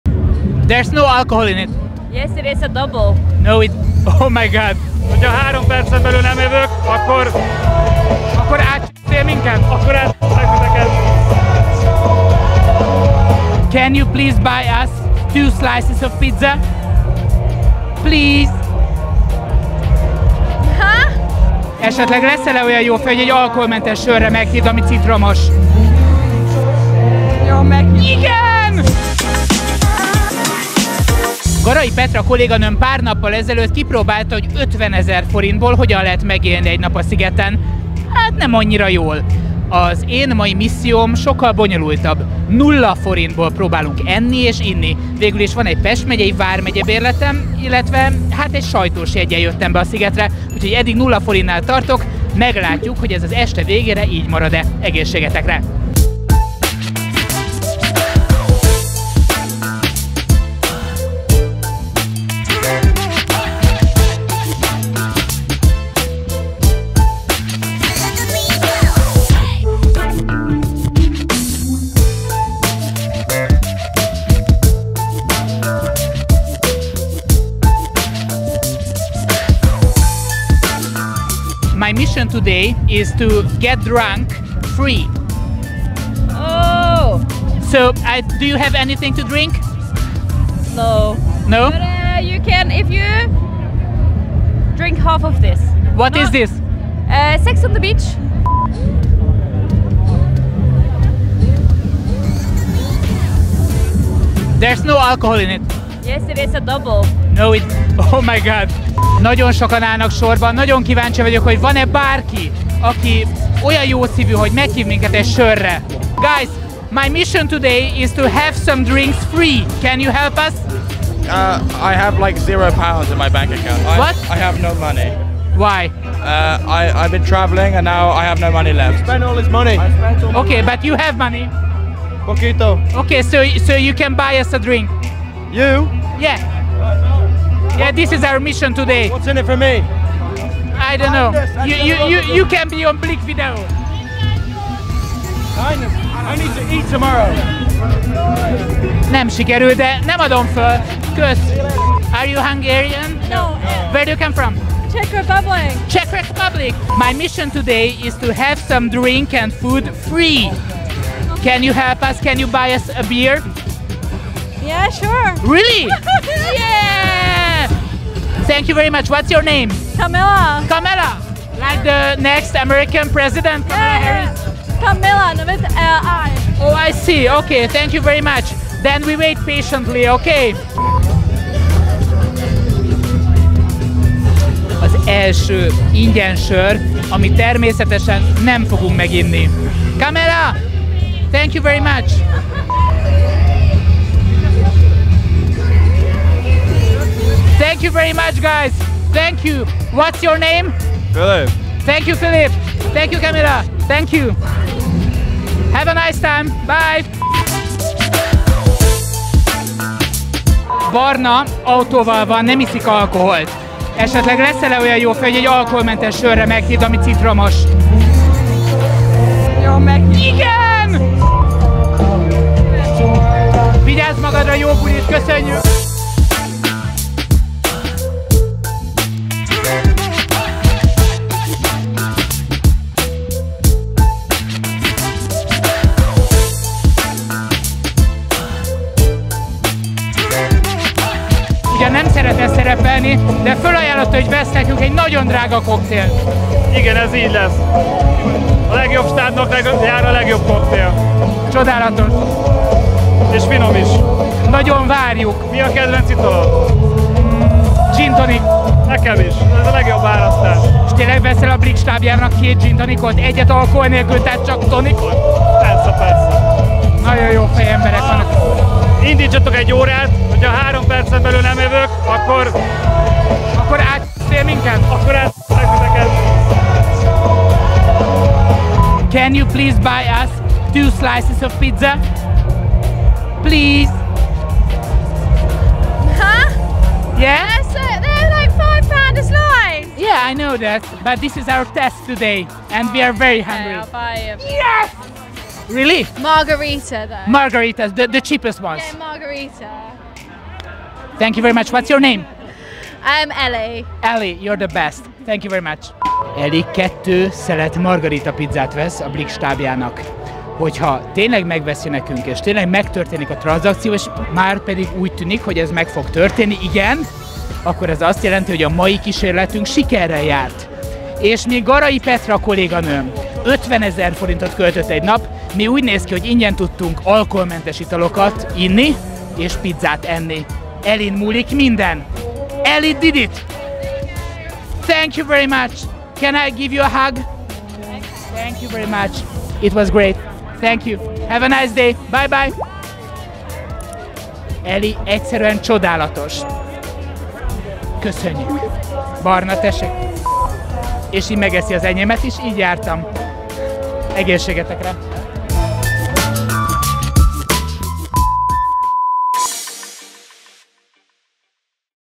Étel és ital 0 forintért a Szigeten? - meghívattuk magunkat - igazi kánaán ingyen
Az idei Sziget fesztiválon pár nappal ezelőtt bemutattuk, mire elég 50 ezer forint a fesztiválon? - Ha levonjuk a napijegy árát, kicsit több, mint 10 ezer forintunk marad, amely a buli közben nem sok mindenre elég. Kolléganőnk azért kávét, hamburgert is vett, sörre hívott meg egy fesztiválózót és még egy műbikát is meglovagolt.

A mostani kihívás pont ennek ellenkezője. A Blikk TV stábja arra vállalkozott, hogy 0 forintból próbál meg jóllakni és itókát vásárolni. Vajon jószívűek voltak a bulizók és a kedvünkért odatartották bankkártyájuk a kártyaolvasóhoz?